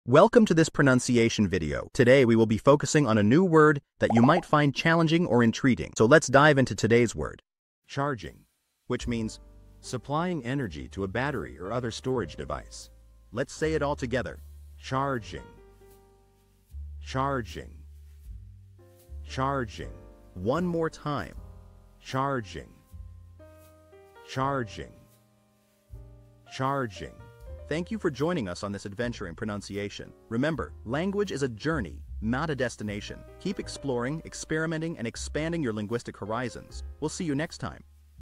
How to pronounce Charging
Master the Pronunciation of 'Charging' - which means : Supplying energy to a battery or other storage device. 📝 with @PPrononciation 🎓🗣️ - [Your Guide to English]

Learn to pronounce 'Charging' perfectly in English with our easy and engaging tutorial by @PPrononciation! 📖📽️ In this comprehensive video, we provide you with native speaker examples 🗣️👂 and phonetic breakdowns 🎵 to ensure you get the pronunciation just right.

How to say Charging in English